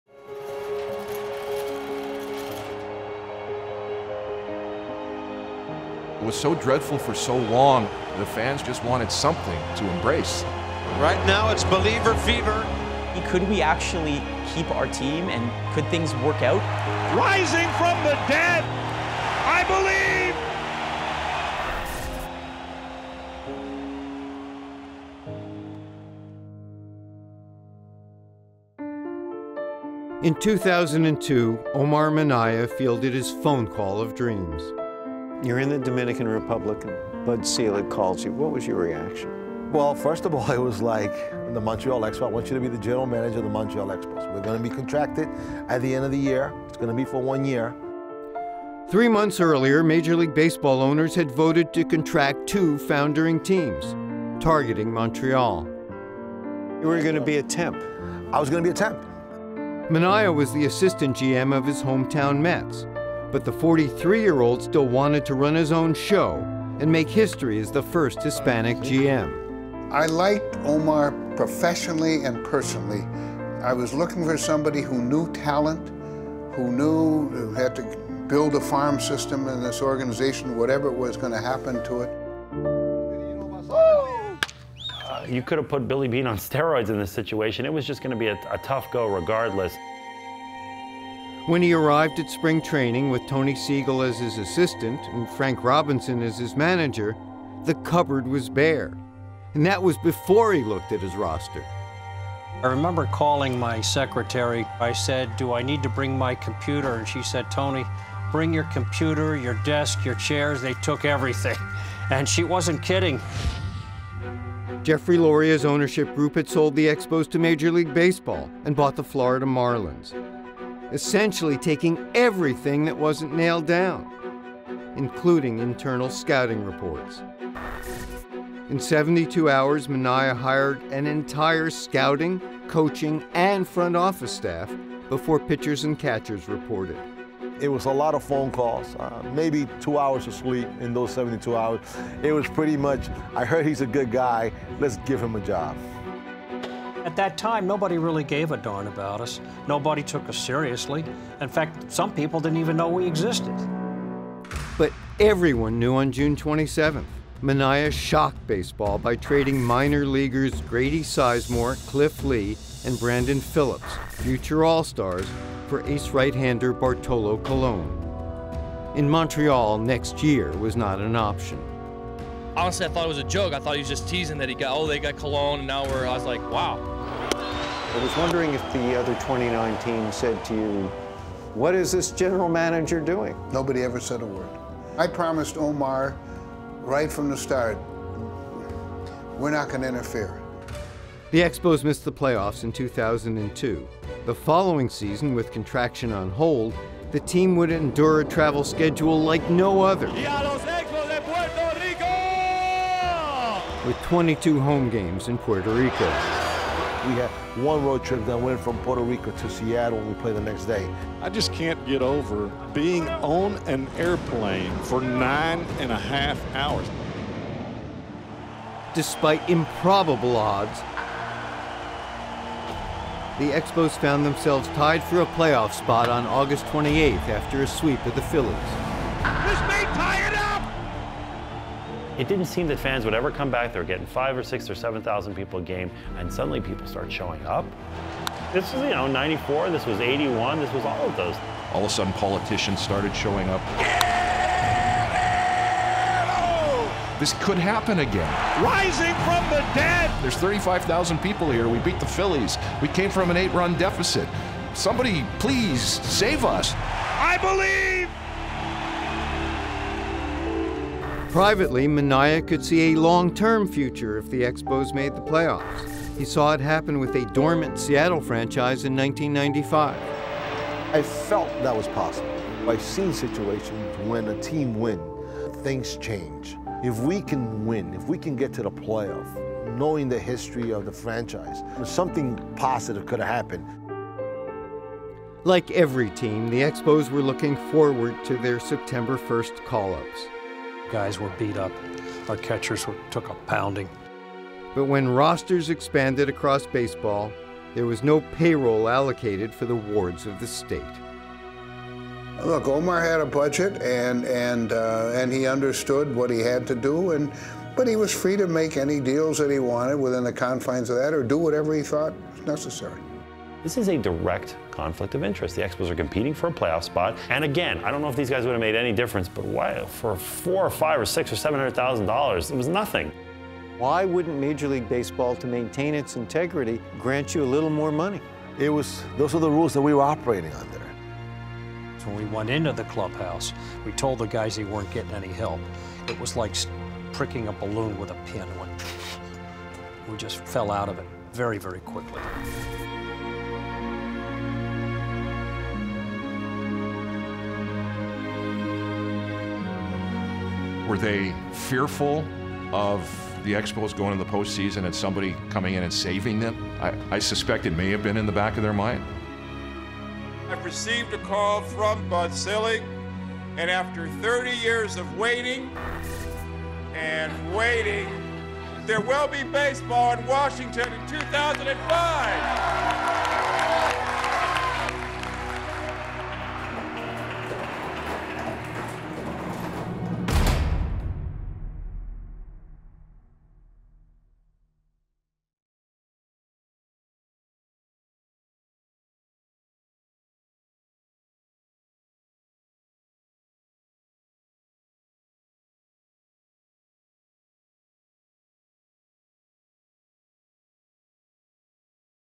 The CONTROVERSIAL ending to the Montreal Expos franchise
On the 20th anniversary of the Montreal Expos' final game, we look back at the road that led to the relocation of Montreal's beloved baseball team.